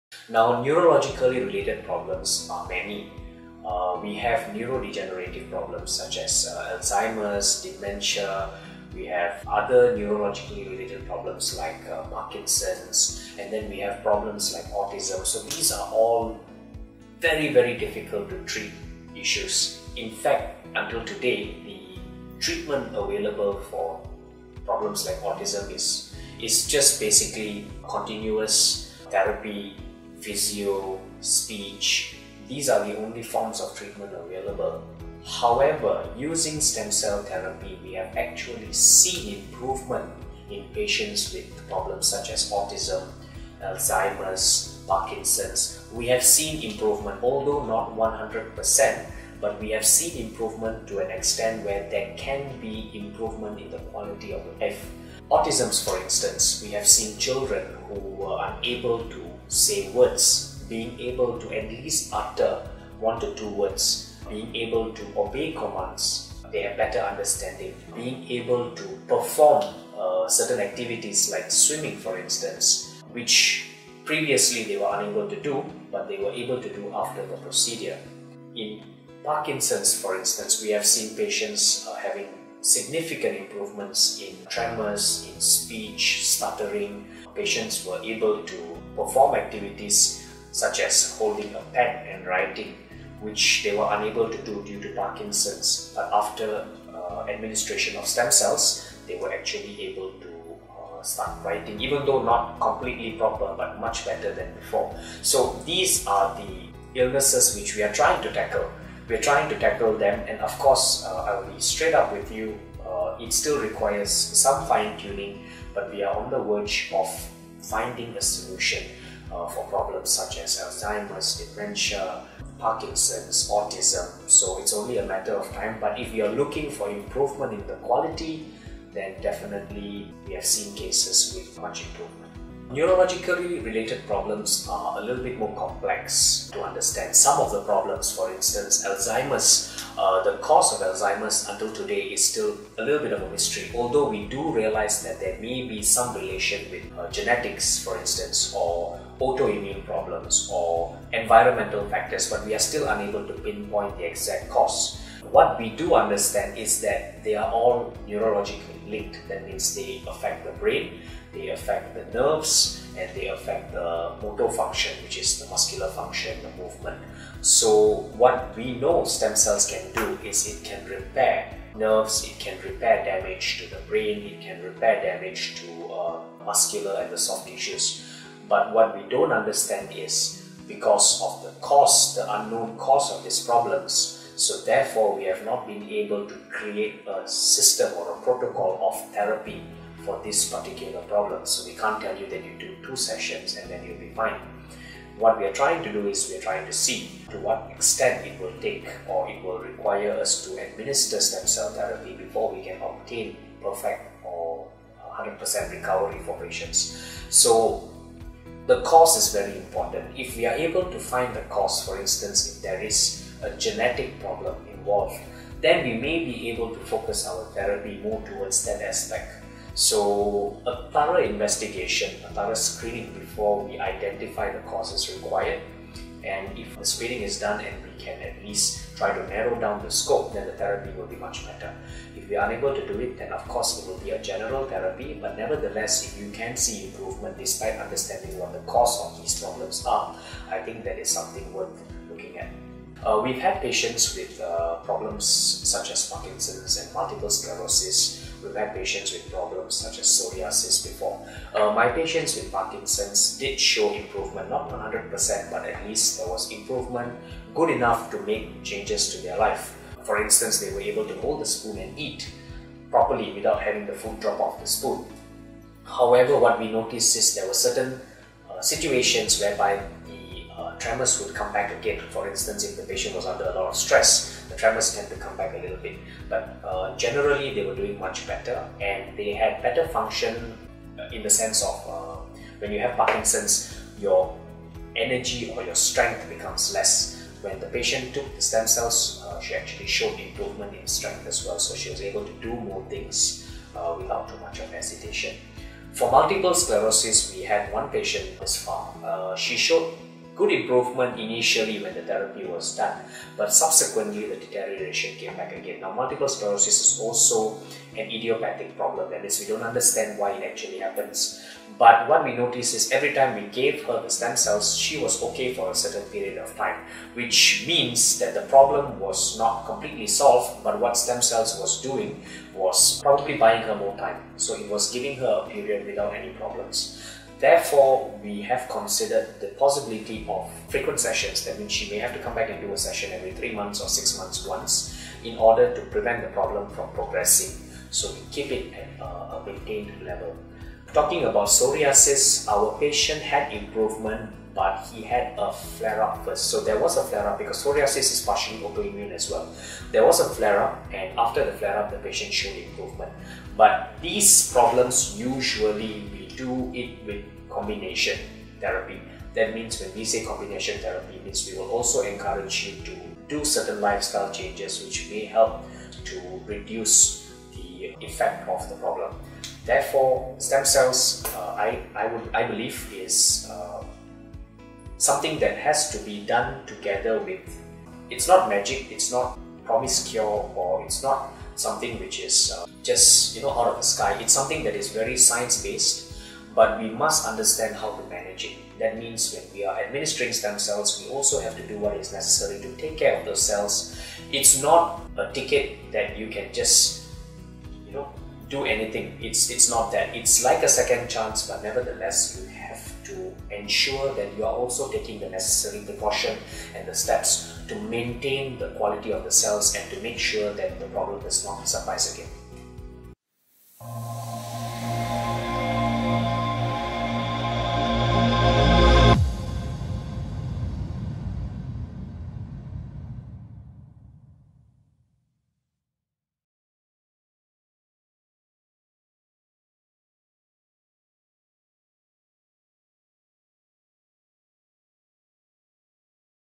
Stem cell therapy for neurological issues (Alzheimer, Parkinson, dementia, autism, psoriasis)
Dr.Rubben talks about how and to what extent mesenchymal stem cells from umbilical cord Wharton Jelly can help with difficult neurological issues like Alzheimer, Parkinson, dementia, autism, psoriasis and more.